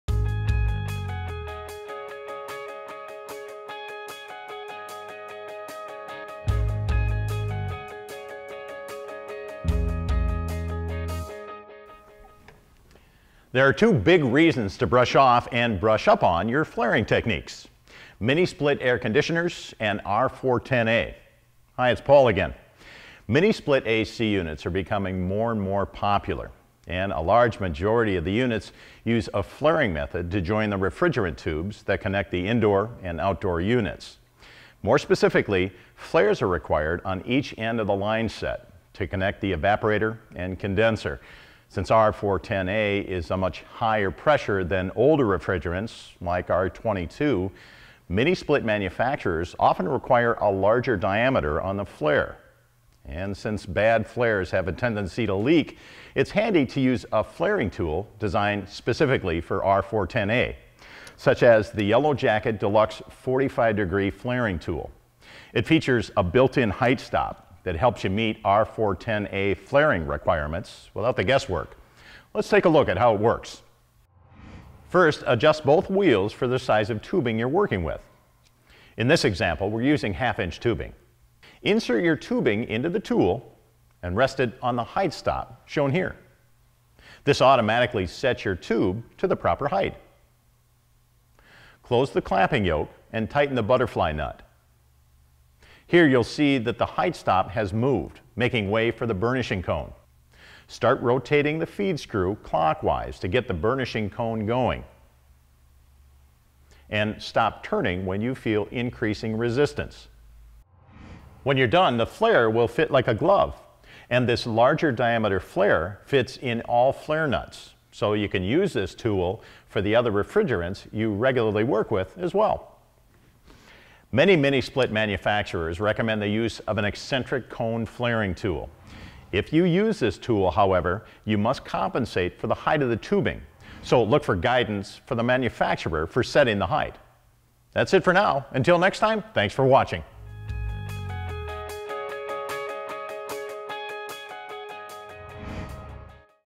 YELLOW JACKET Quick Tip: R-410A, Mini Splits and Flaring
Discover which YELLOW JACKET tool can help you improve your flaring techniques when working with mini splits and R-410A.